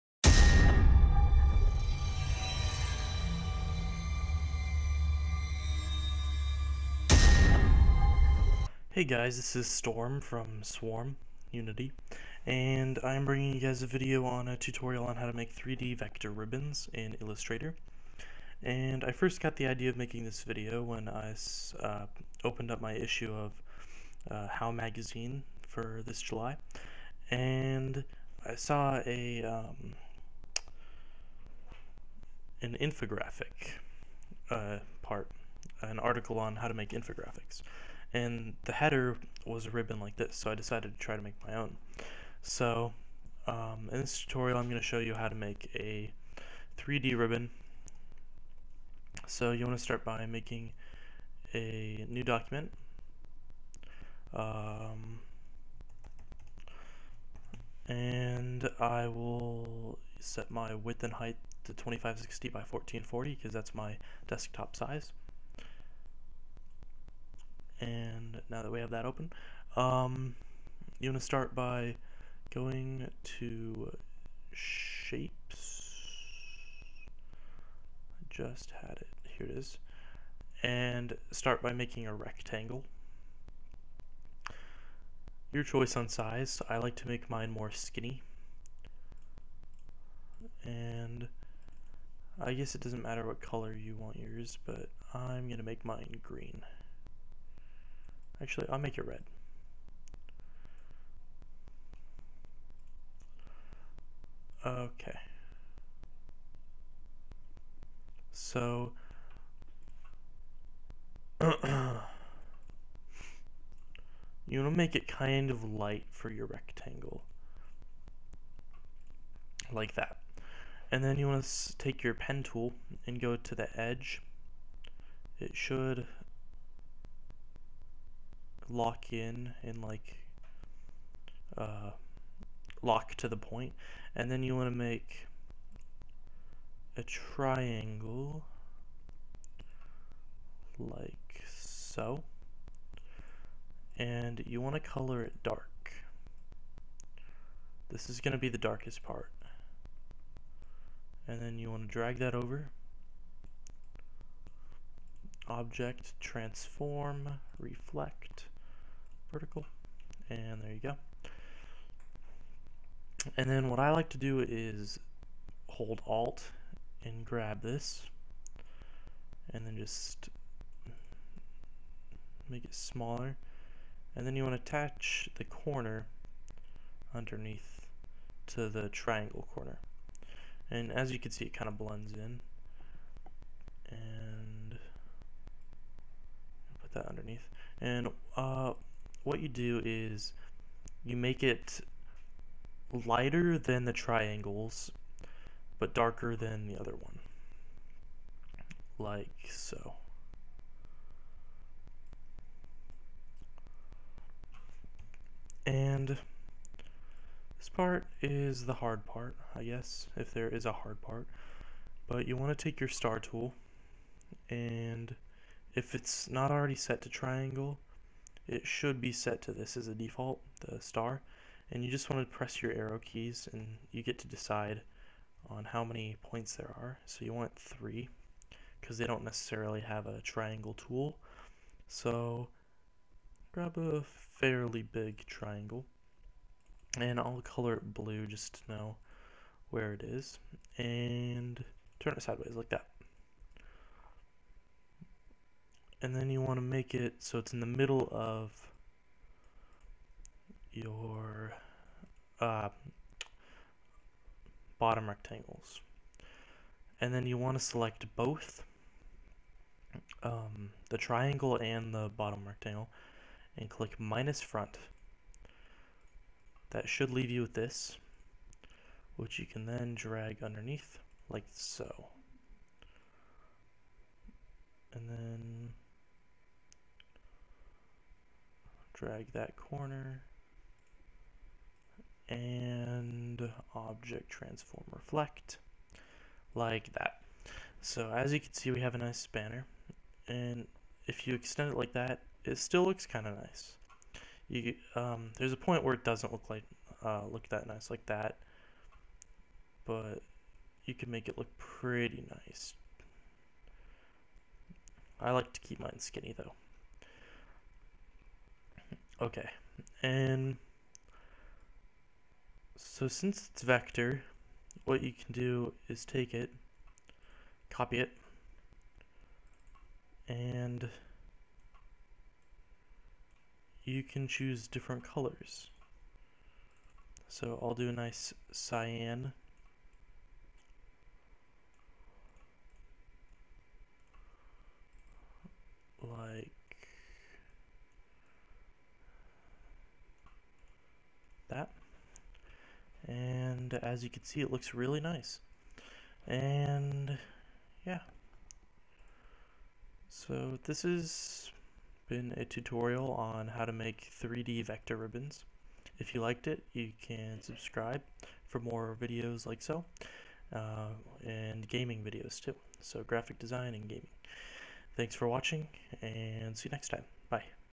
Illustrator Tutorial - Easy 3D Ribbons
In this tutorial, me (storm), will be showing you how to make basic 3D ribbons. If you are to lazy to make some, here is a pack I made: